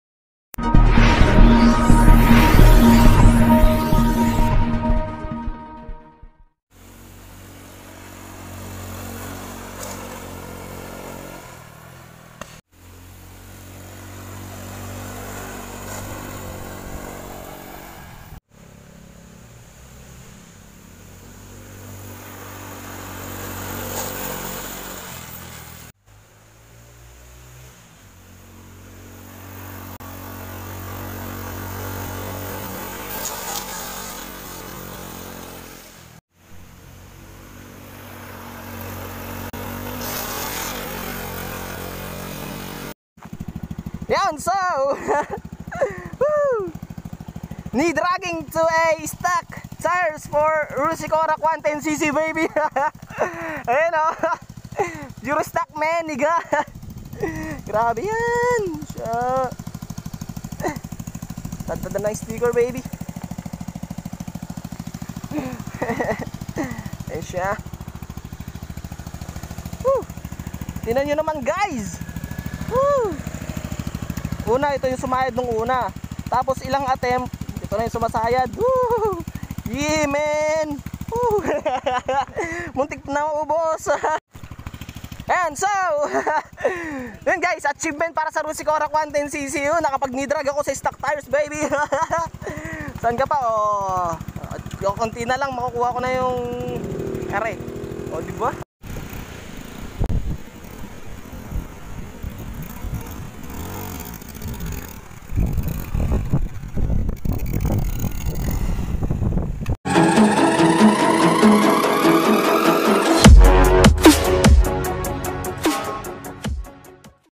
Knee Dragging on a Rusi korak 110cc / AioriMoto
Gears :
Jacket
Pro racer elbow & knee pads
Fox gloves
Sec Helmet
Oppo A3s camera
Rusi korak 110cc bike

Ride safe always!